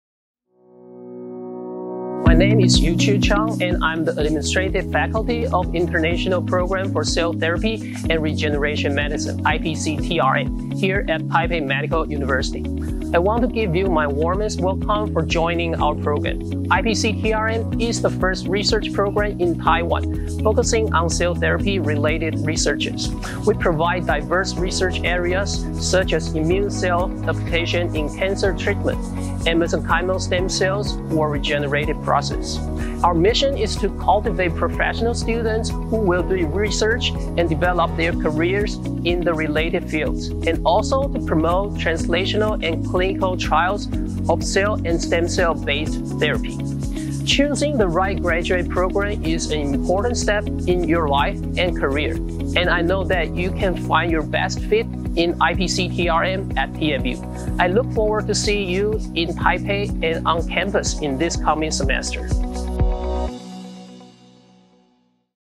Welcome to International PH.D. Program for Cell Therapy and Regeneration Medicine
Assistant Professor Chang, Yu-Chu on behalf of IPCTRM to welcome you all to Taipei Medical University.